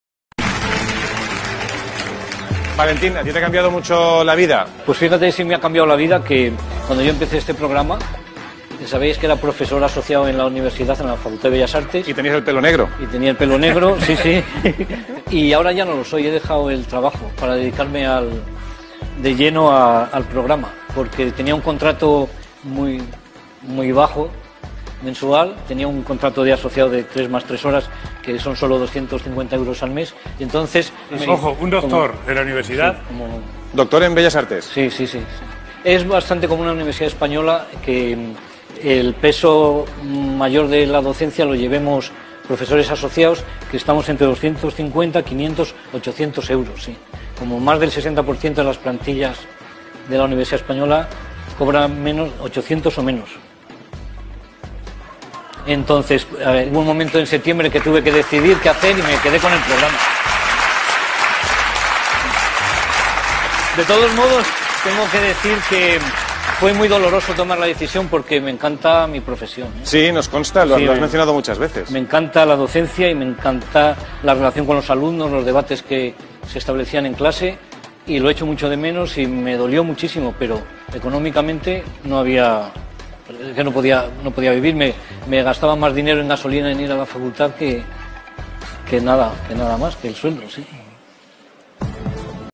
Un profesor de Universidad deja su trabajo por un concurso en la tele
Video meramente informativo, subido sin animo de lucro